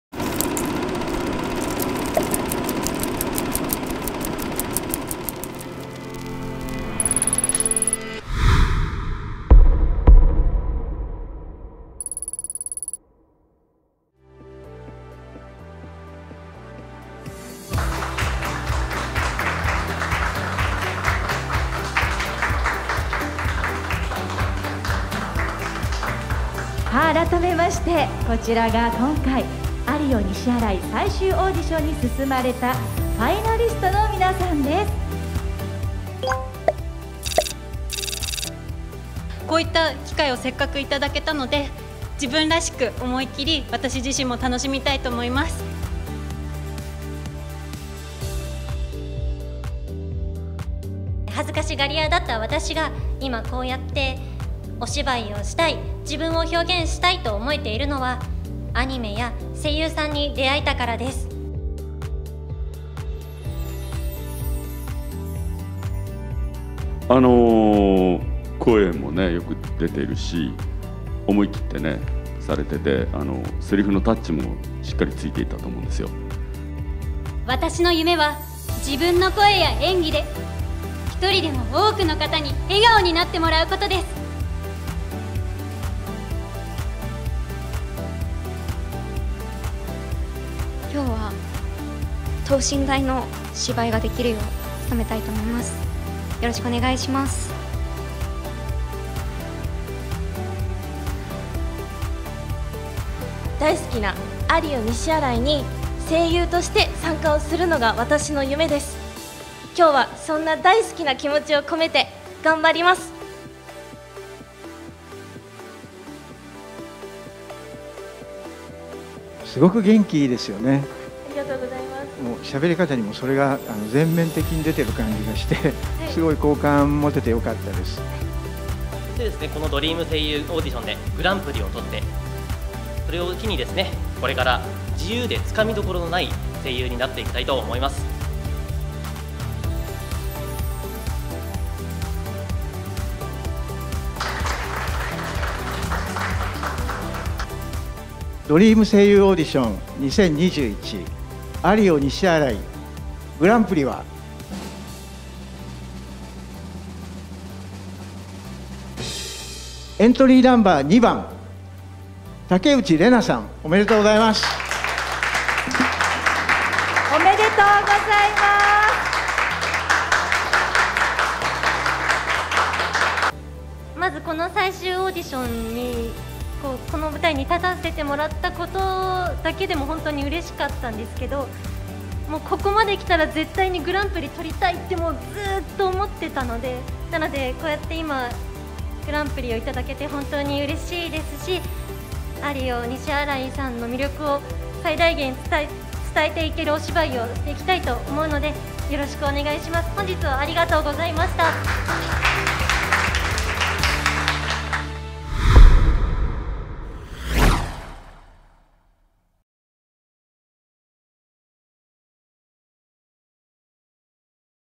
ドリーム声優オーディション2021 アリオ西新井 最終オーディション
本動画は、ドリーム声優オーディション2021（アリオ西新井）のファイナリストオーディション動画です。